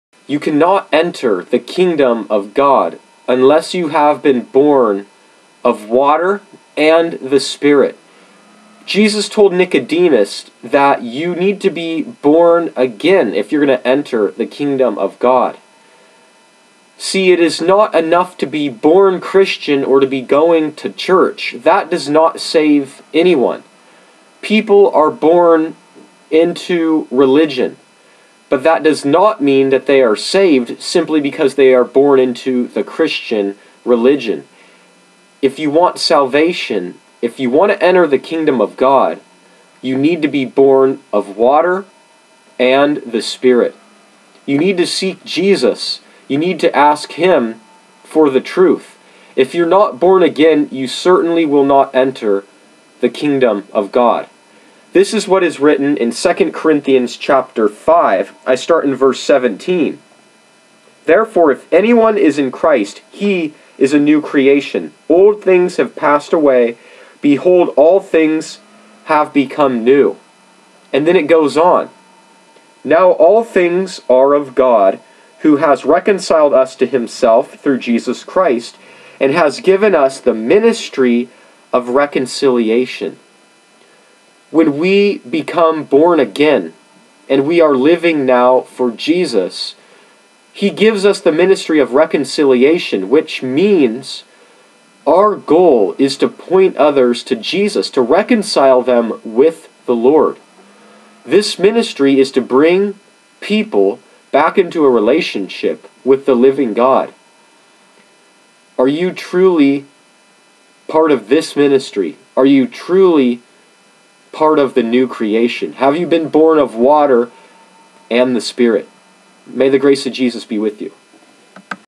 Have You Been Born AGAIN?
You cannot enter the Kingdom of God unless you have been born of water and the Spirit.  Jesus told Nicodemus that you need to be born again if you going to enter the Kingdom of God.   See, it is not enough to be born Christian or to be going to church, that does not save anyone.  People are born into religion, but that does not mean that they are saved simply because they are born into the "Christian" religion.  If you want salvation, if you want to enter the Kingdom of God, you need to be born of water AND the Spirit.  You need to seek Jesus!  You need to ask HIM for the truth.  If you're not born again you certainly will not enter the Kingdom of God.   This is what is written in 2 Corinthians chapter 5. I start in verse 17.  "Therefore, if anyone is in Christ, he is a NEW creation; old things have passed away; behold, all things have become new."  And then it goes on... "Now ALL things are of God who has reconciled us to HIMSELF through Jesus Christ, and has given us the ministry of reconciliation..."  When we become born again and we are living now for Jesus, He gives us the ministry of reconciliation, WHICH MEANS our goal is to point others to Jesus to reconcile them with the Lord.  This ministry is to bring people back into a relationship with the Living God!  Are you TRULY part of THIS ministry?  Are you TRULY part of the NEW creation?  Have you been bon of water AND THE SPIRIT?  May the grace of Jesus be with you.